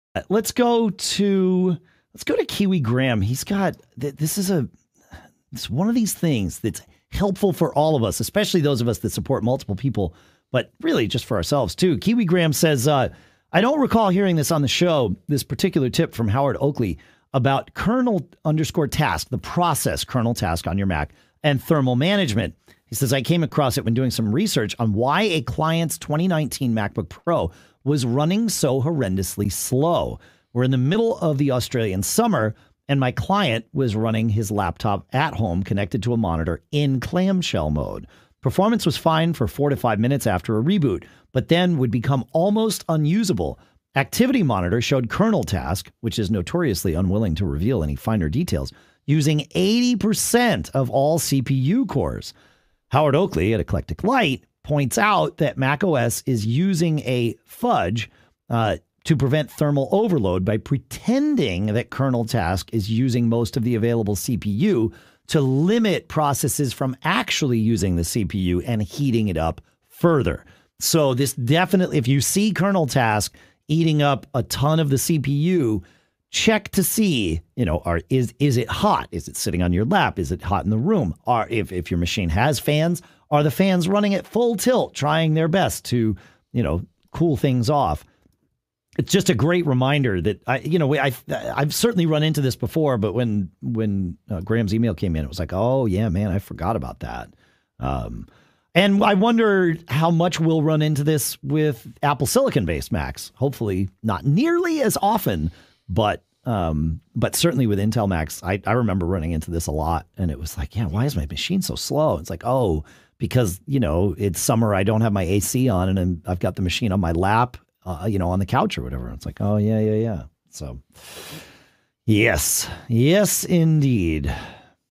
Fixing Kernel_Task High CPU Usage
Here's a quick tip on what you can do to fix kernel_task eating up your CPU.
Your Mac may be overheating!